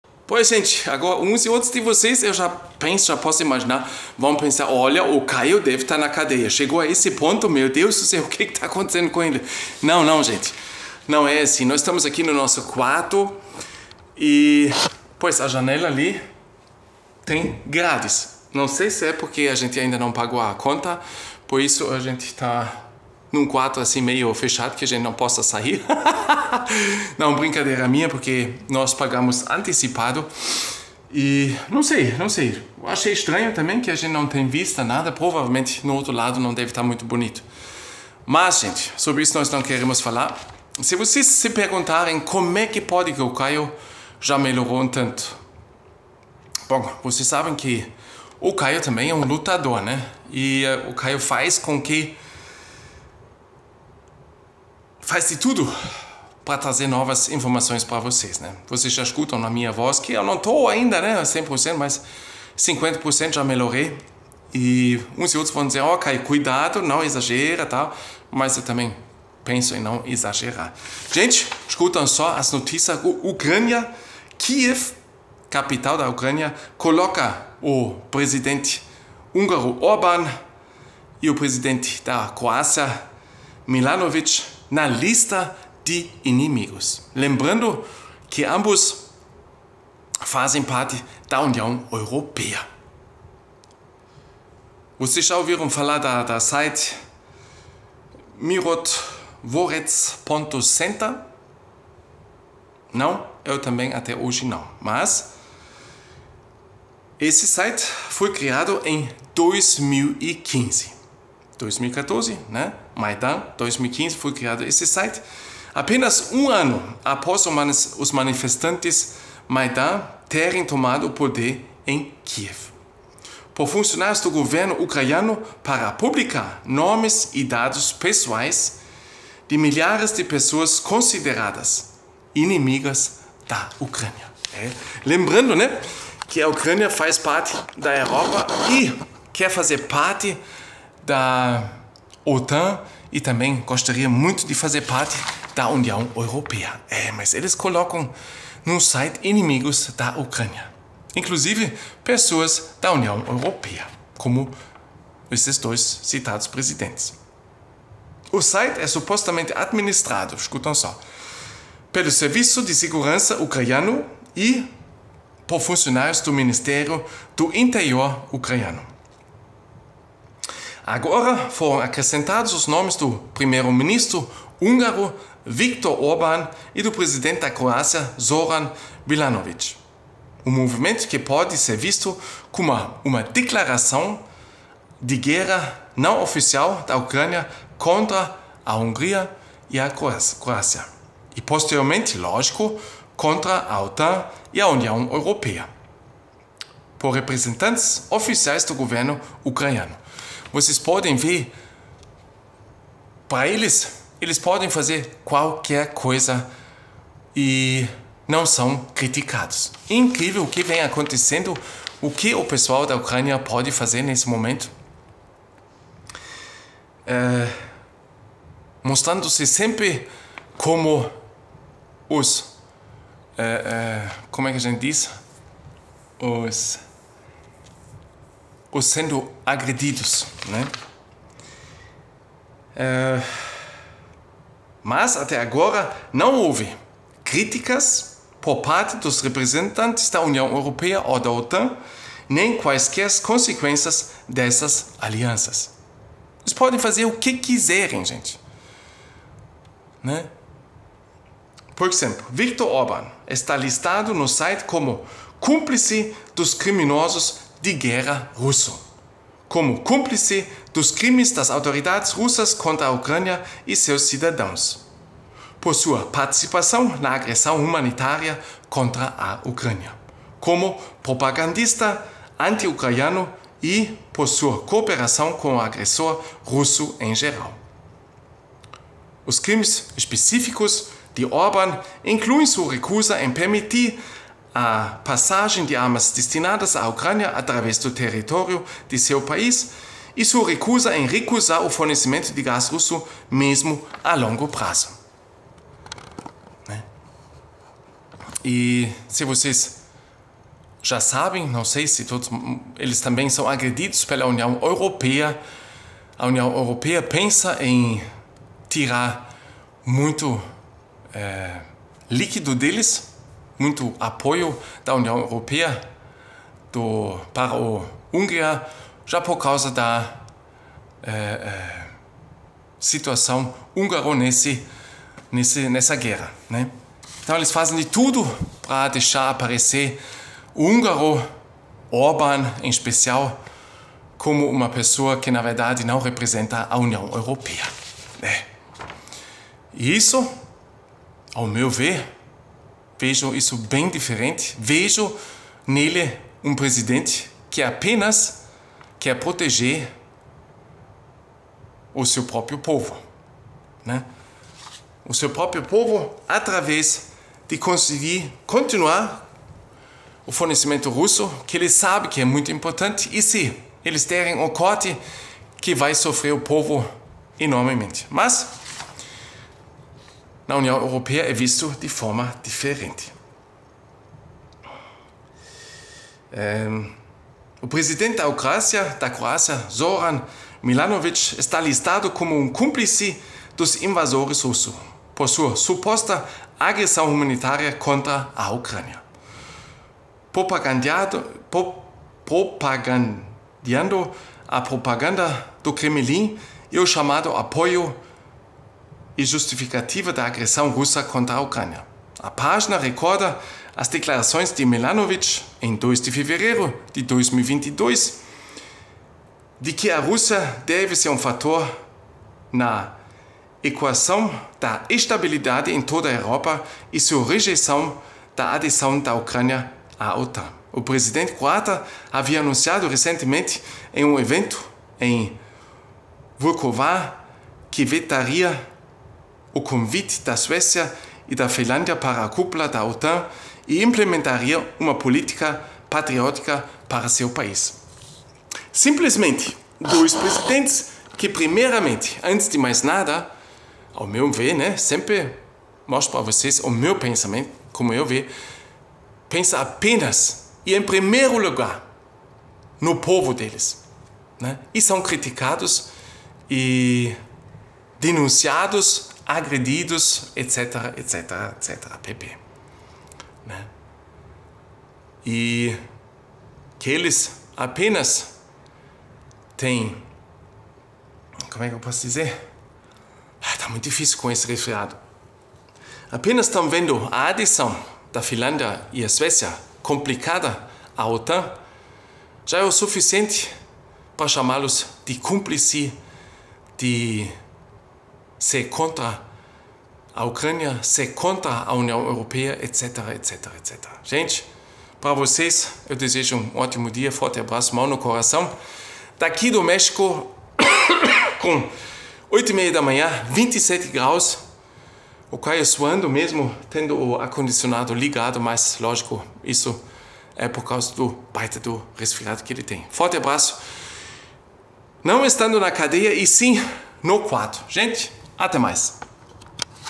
UCRANIA PODE TUDO : COLOCA PRESIDENTE ORBAN E MILANCOVIC NA LISTA DE INIMIGOS !!!!!
NOVO CANAL : O ALEMAO MAIS BRASILEIRO